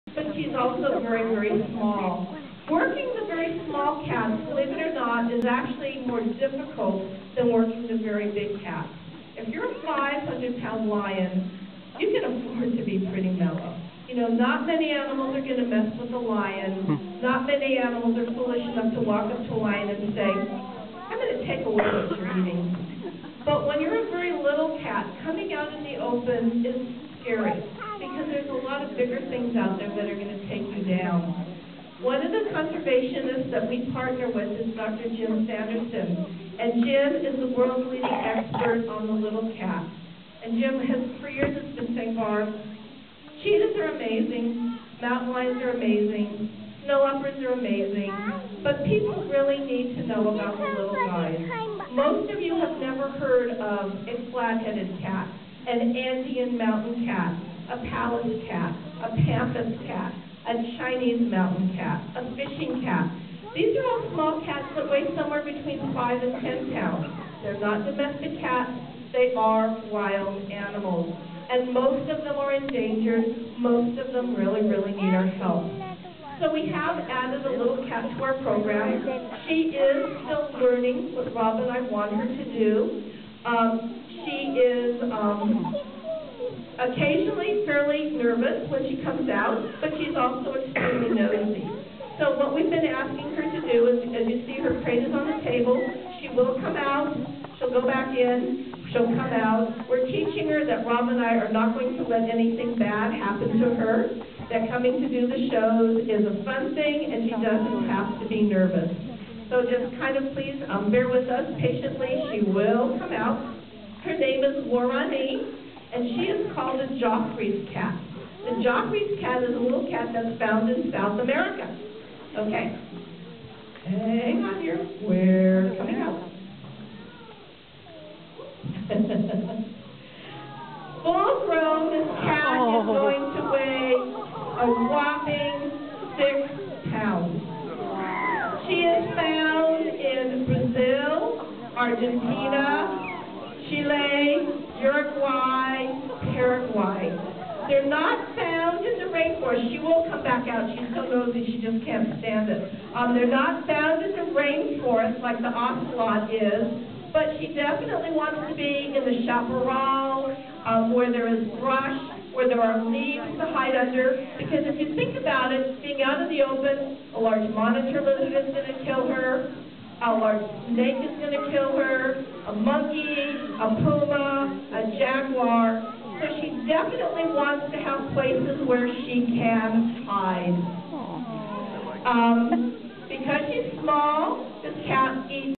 Wildcat Fund Presentation Part 6
The Wildcat Fund is an educational conservation society dedicated to engendering in the public a great love for wildcats throughout the world, and therefore a desire to save them from impending extinction.  I filmed a show of theirs in Santa Cruz, California this February.

This time it's Laurani, the very cute (and very tiny) Geoffrey's Cat.  I apologize that this video cuts off, my camera card filled at that moment and I had to switch to another.